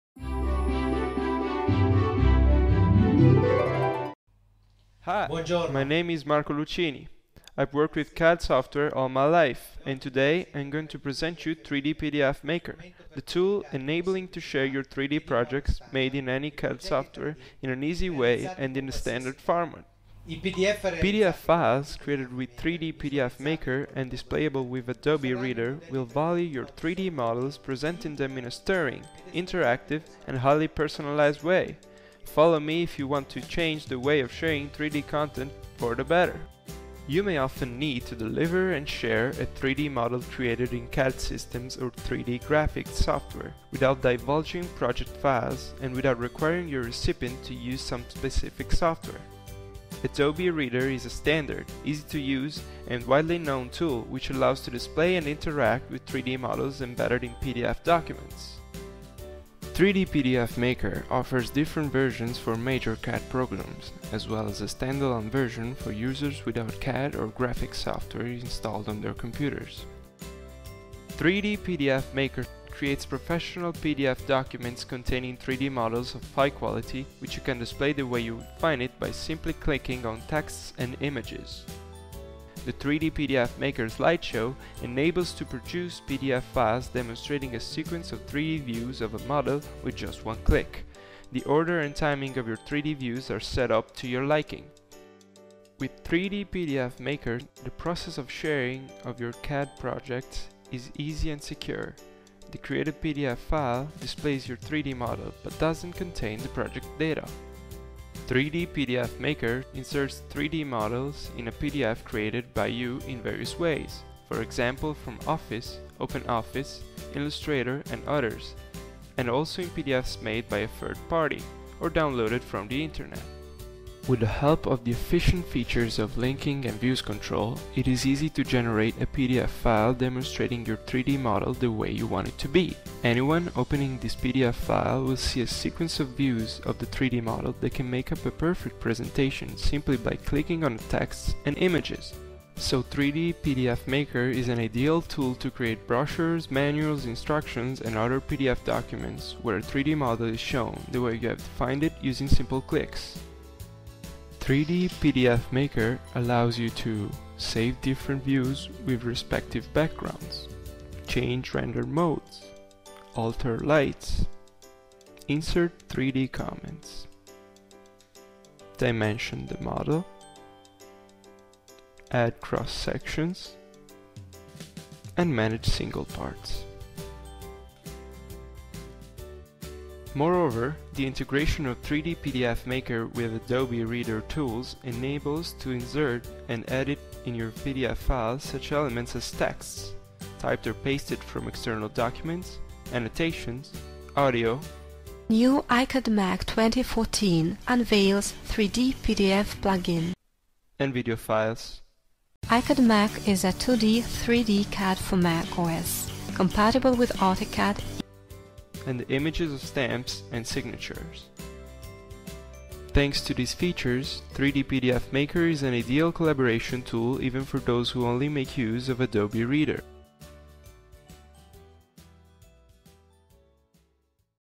3D PDF Maker - The New Way of Sharing 3D Models
PDF files created with 3D PDF Maker and displayable with Adobe Reader will value your 3D models presenting them in a stirring, interactive and highly personalized way.
3D PDF Maker offers different versions for major CAD programs as well as a Standalone version for users without CAD or graphics software installed on their computers.